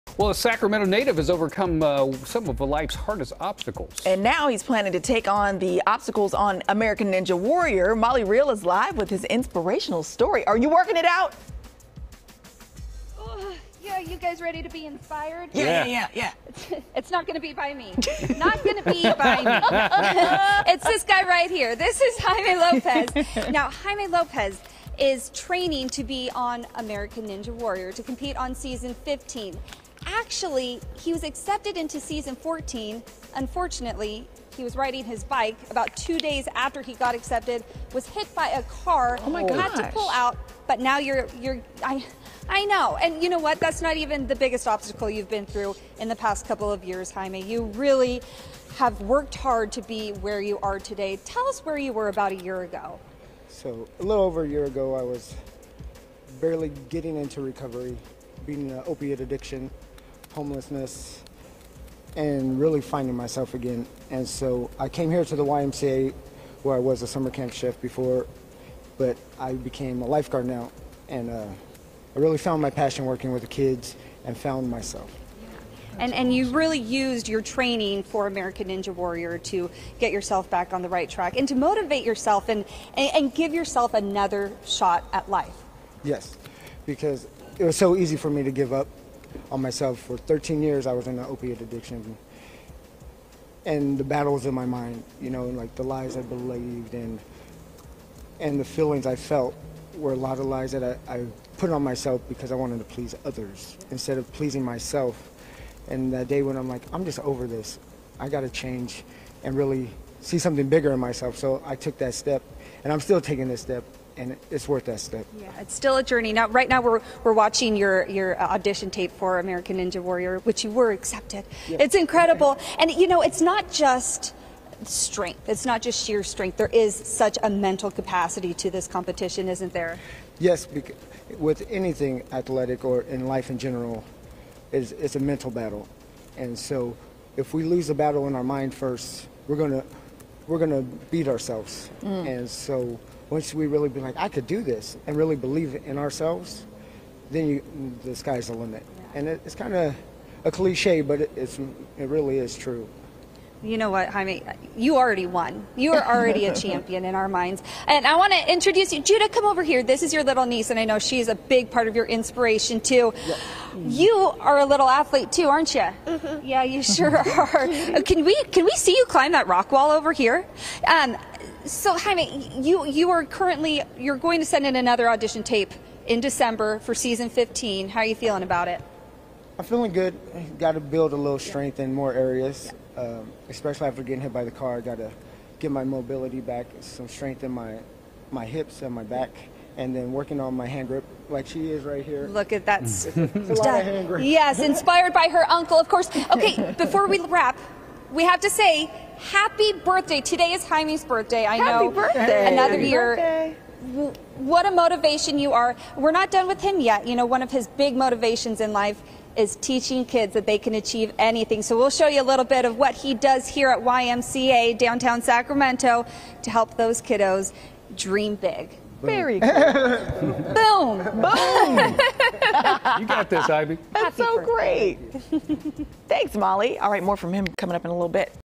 Local "American Ninja Warrior" Hopeful Jaime Lopez!
Sacramento native Jaime Lopez has overcome homelessness and addiction and is now hoping to compete on American Ninja Warrior. He used this as motivation to stay sober and get his life back. Accepted to Season 14, he had to give up his spot when he was hit by a car while riding his bike. He's training for Season 15 now, and Molly Riehl is finding out more about Jaime's quest!